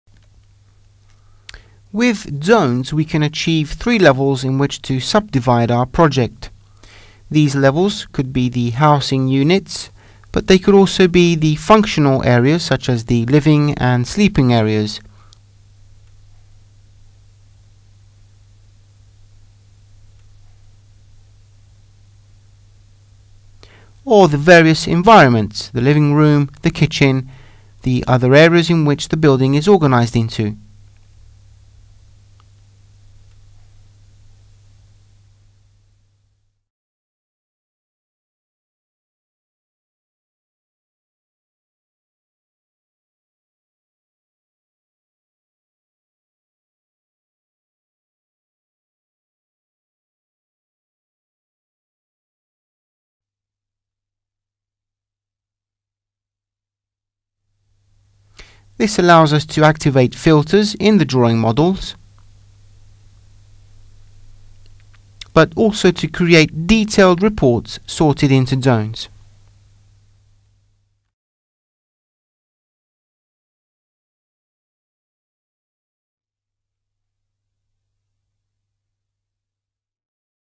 Edificius Tutorial - Zones - ACCA software
See how easy it is to deal with Defining the areas in which to divide the project using a BIM software. Learn more about Defining the areas in which to divide the project in a 3D BIM model the easy and quick way. Find out more about Edificius and download the Trial version and discover its powerful BIM design tools .